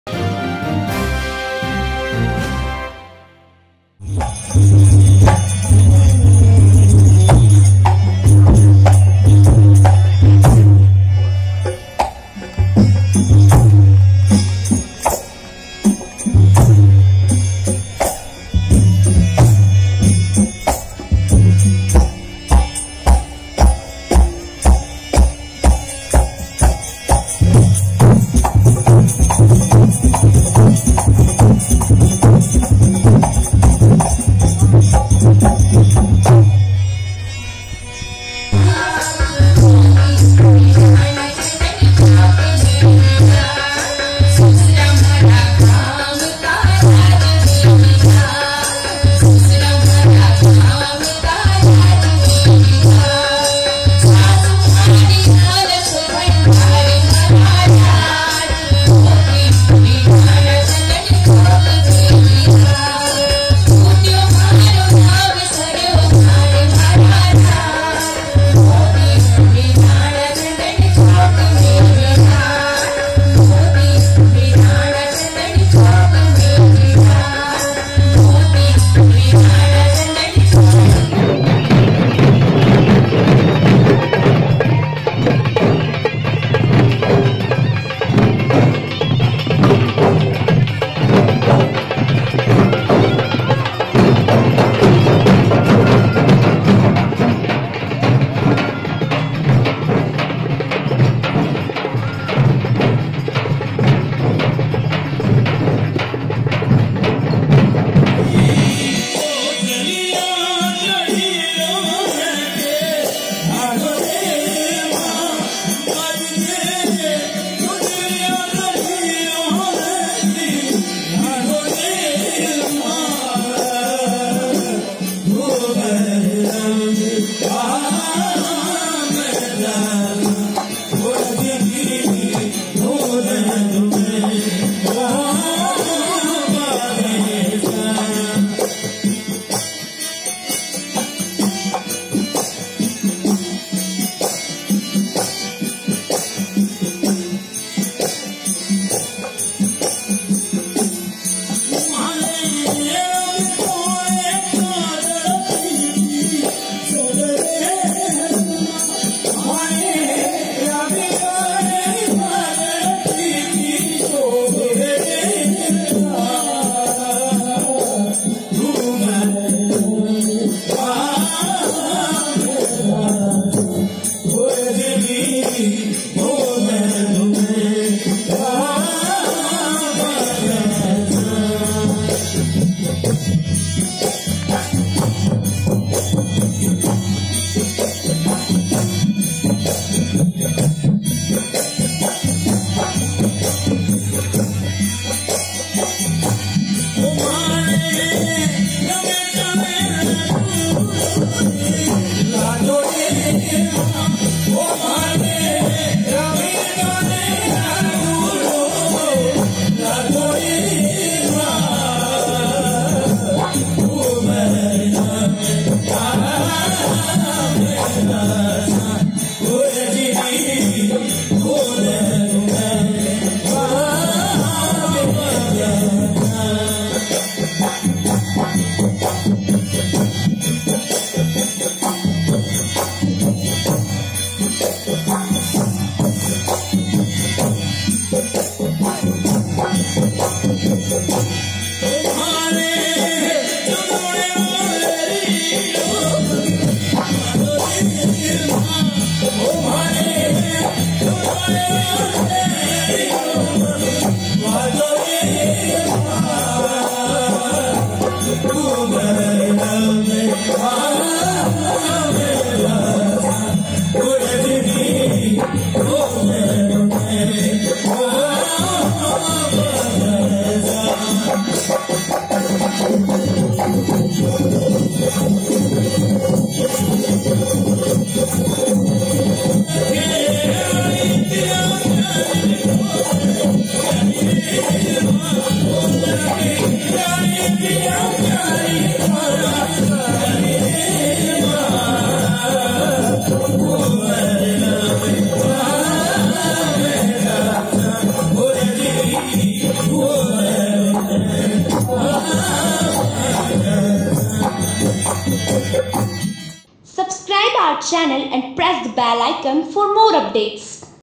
इंदौर के प्रसिद्ध मालवा उत्सव में हुए अनूठे नृत्य
मन मोह लेंगे  ये लोकनृत्य आपका